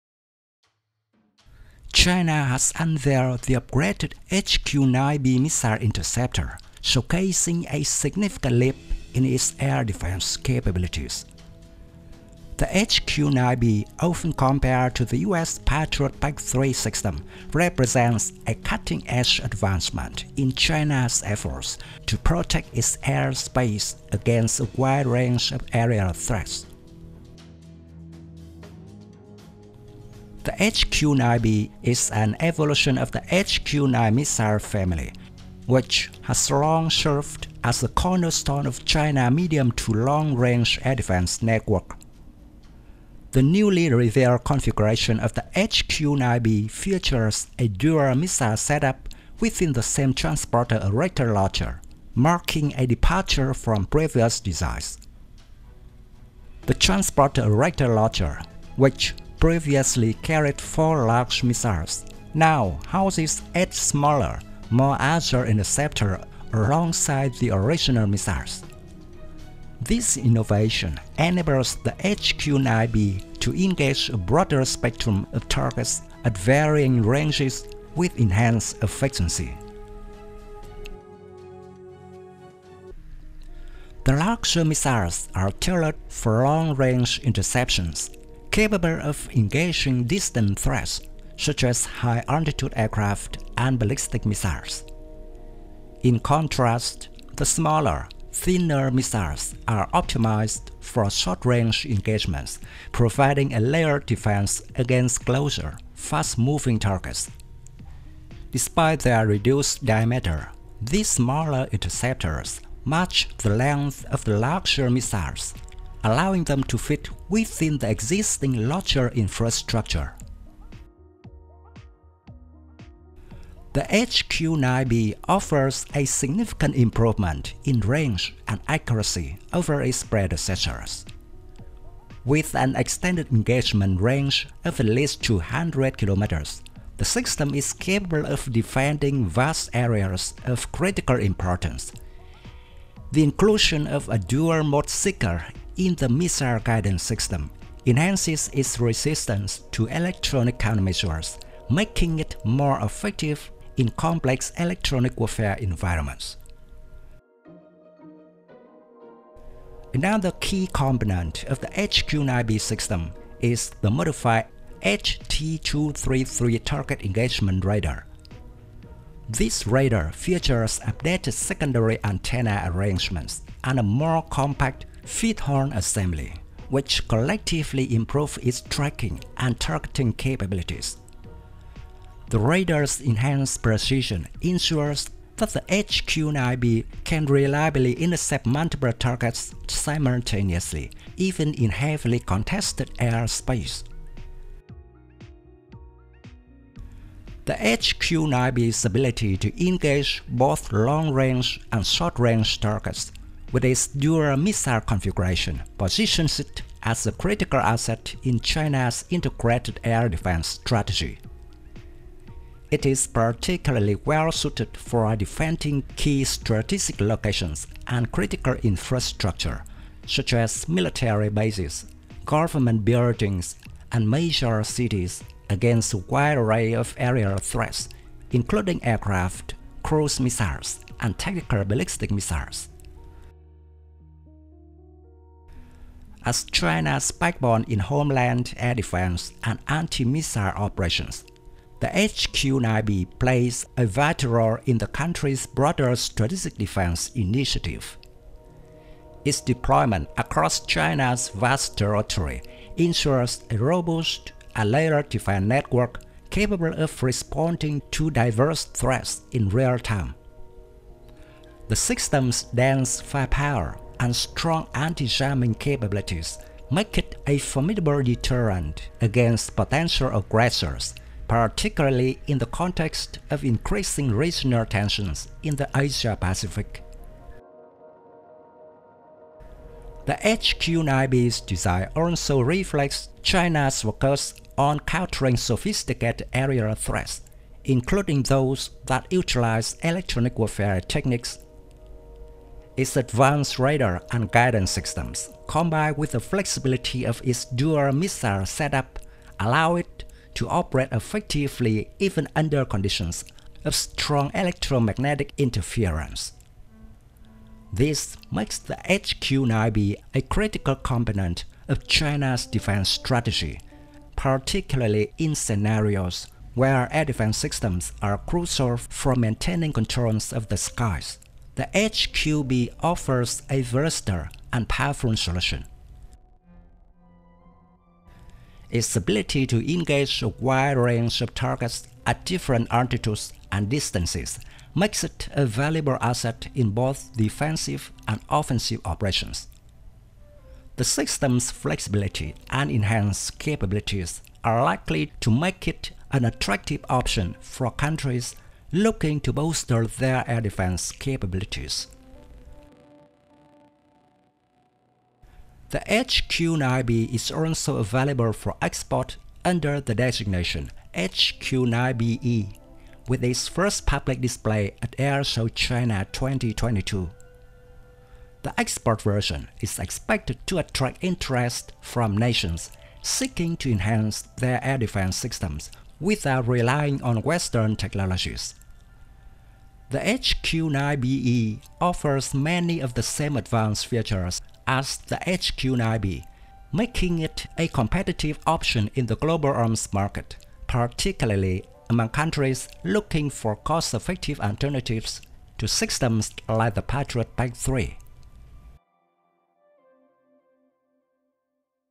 China's HQ-9B: A New Era in Advanced Missile Defense Technology
Explore China's latest leap in air defense with the HQ-9B missile interceptor. This upgraded system marks a significant advancement in China's efforts to secure its airspace, rivaling systems like the U.S. Patriot PAC-3. The HQ-9B, an evolution of the HQ-9 family, features a groundbreaking dual-missile setup within a single launcher, allowing it to engage a broader range of threats with enhanced efficiency. With extended range, improved accuracy, and advanced electronic warfare capabilities, the HQ-9B is poised to play a pivotal role in modern warfare and the global arms market.

Thank you very much.
Best regards
Dung Tran.